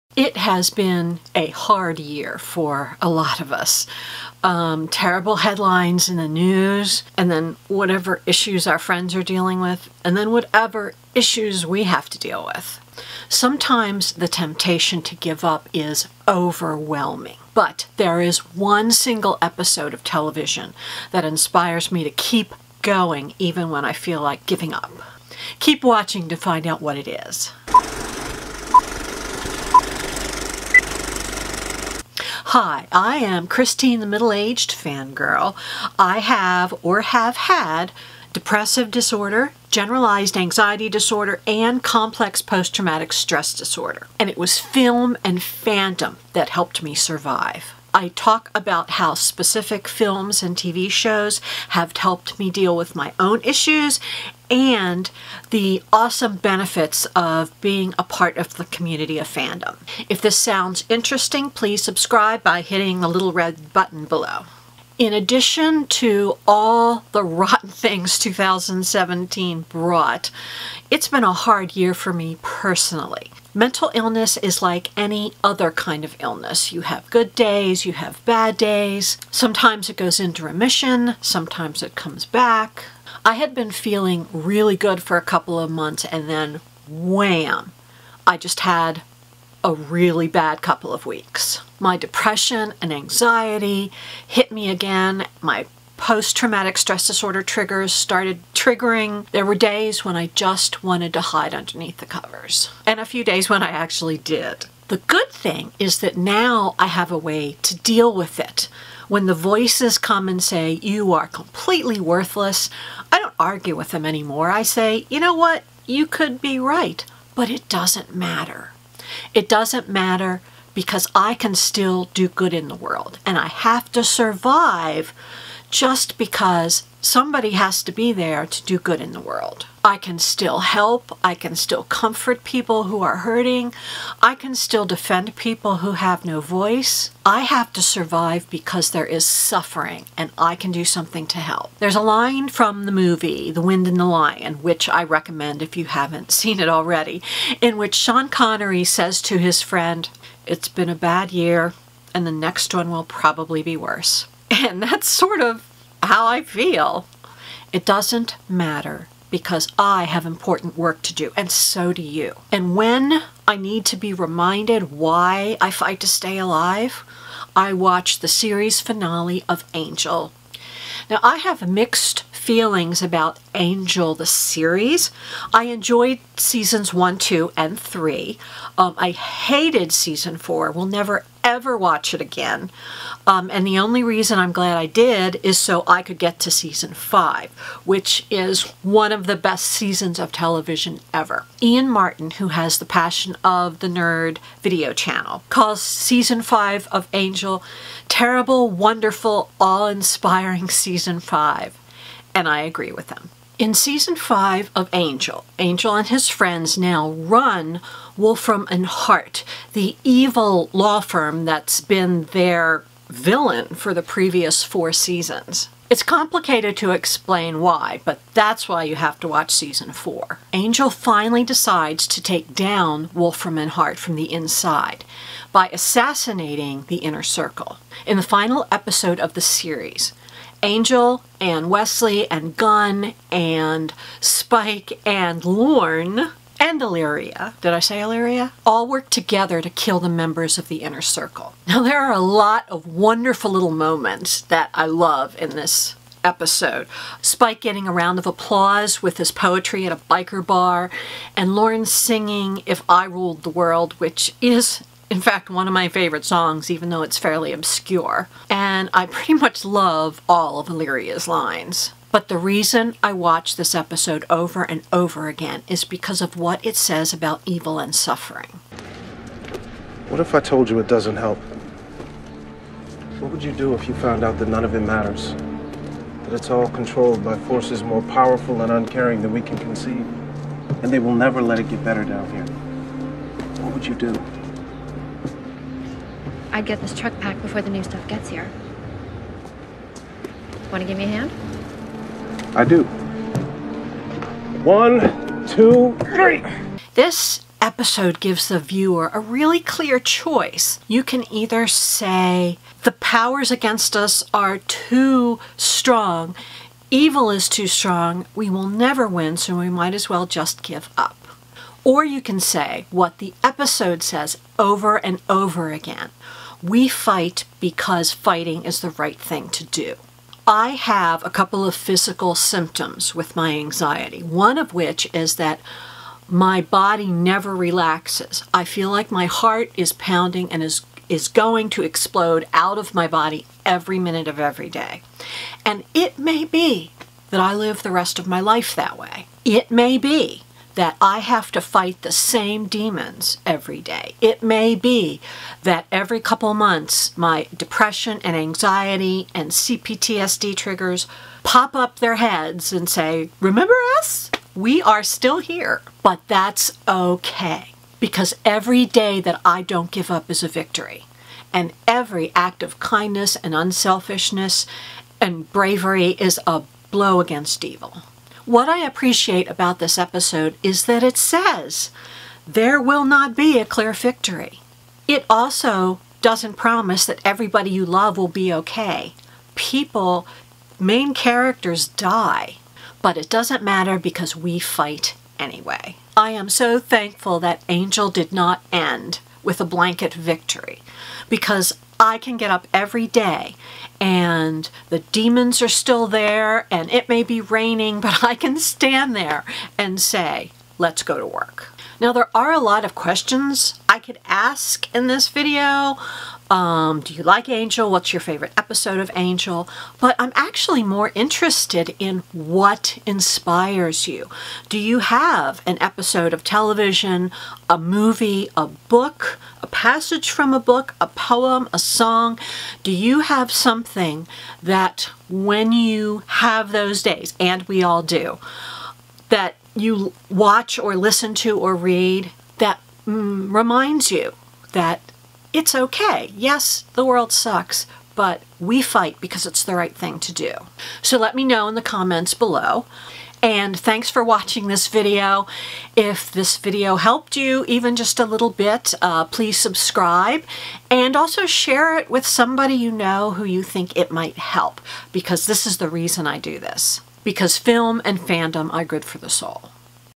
How Angel inspires me when I feel like giving up
Sometimes it feels like the world sucks. But the series finale of "Angel" inspires me to keep fighting even it seems like evil is winning.